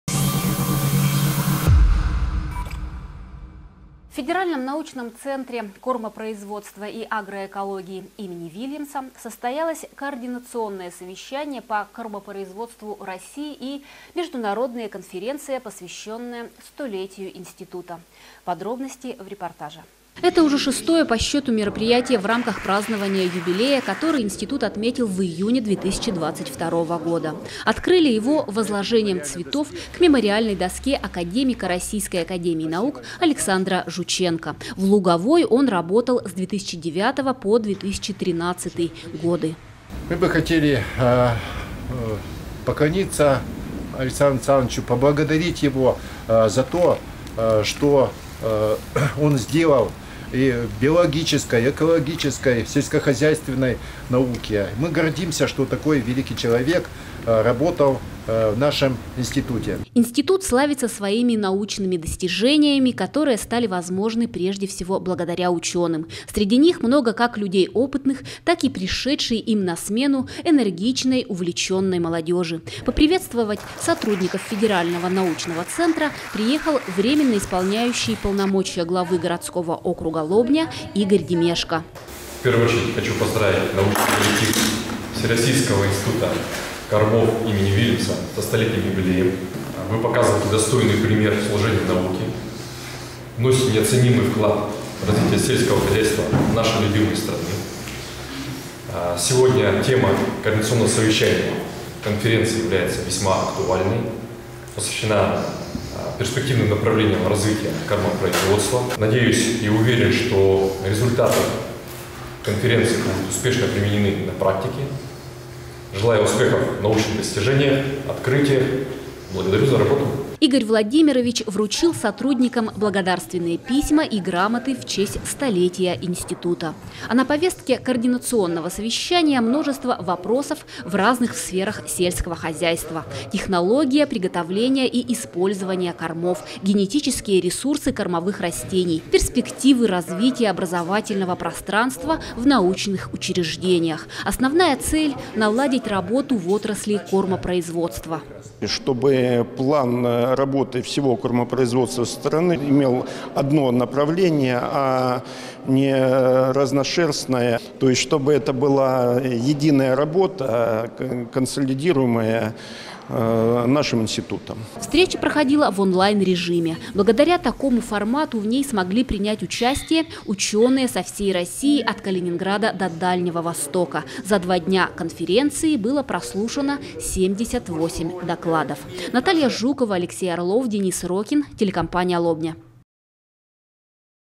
Международная конференция в ВИК им. В.Вильямса
В федеральном научном центре кормопроизводства и агроэкологии им. Вильямса состоялось координационное совещание по кормопроизводству России и международная конференция посвященная 100-летию института. Подробности в репортаже.